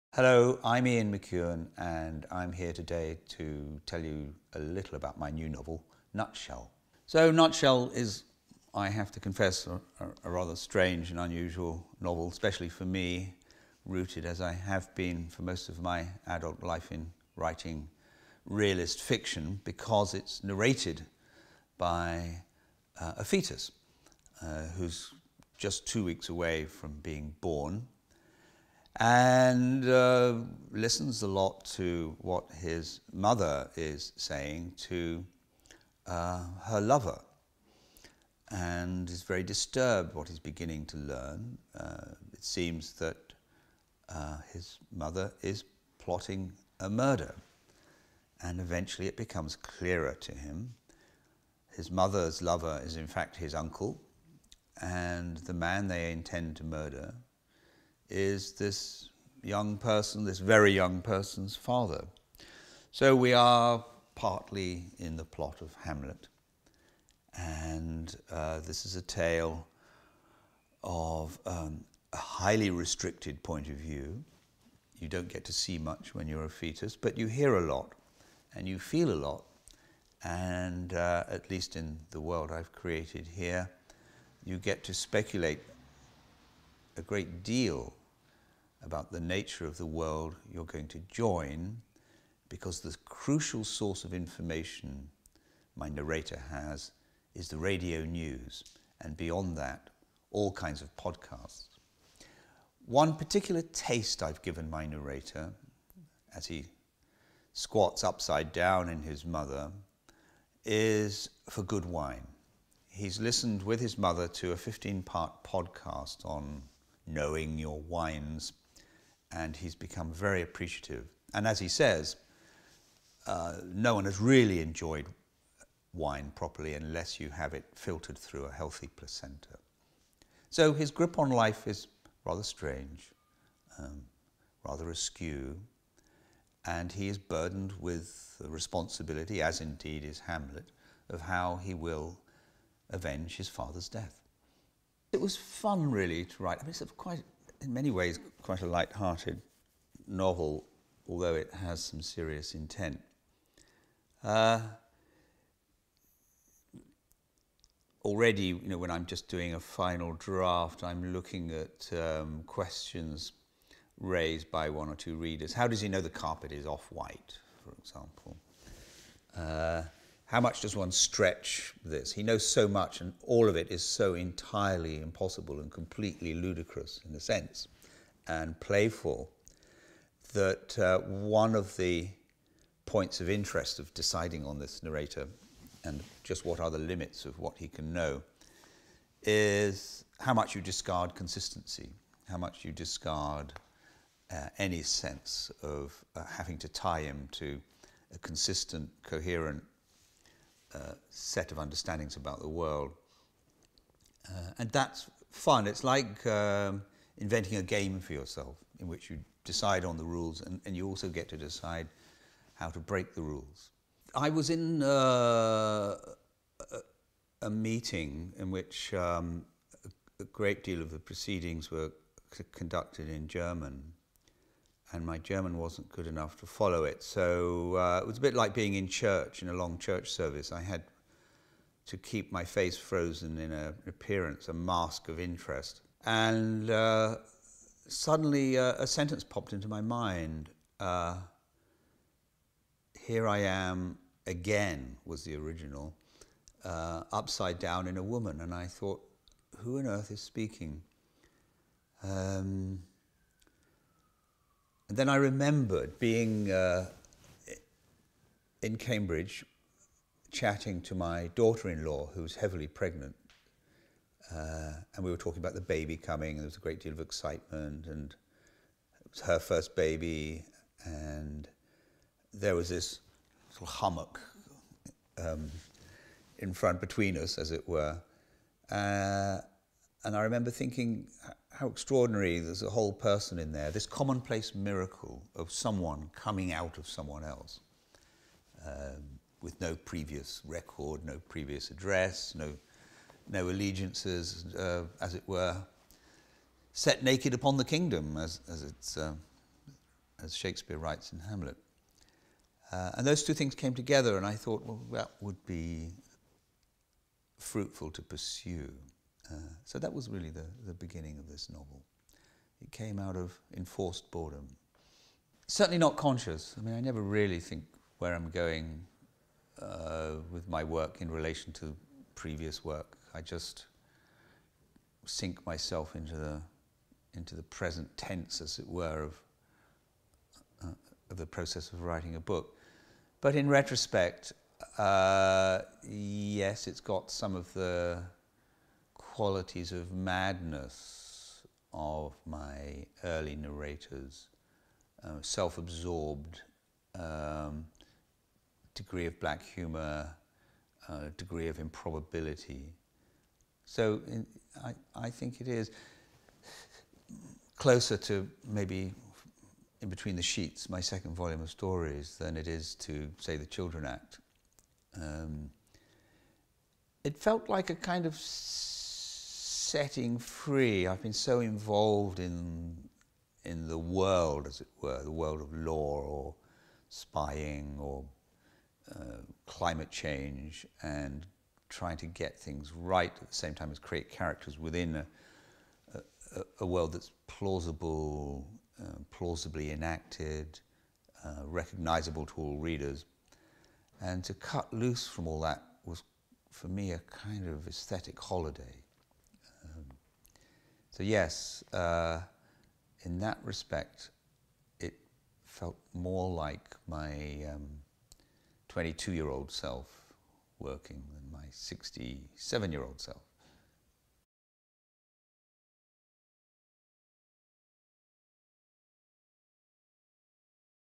Ian McEwan on 'Nutshell'
Nutshell is an altogether original story of deceit and murder, told by a narrator with a perspective and voice unlike any in recent literature. Love and betrayal, life and death come together in the most unexpected, moving ways in this sensational new novel from Ian McEwan, which will make readers first gasp with astonishment then laugh with delight. Dazzling, funny and audacious, it is the finest recent work from a true master, beautifully told, brilliantly executed.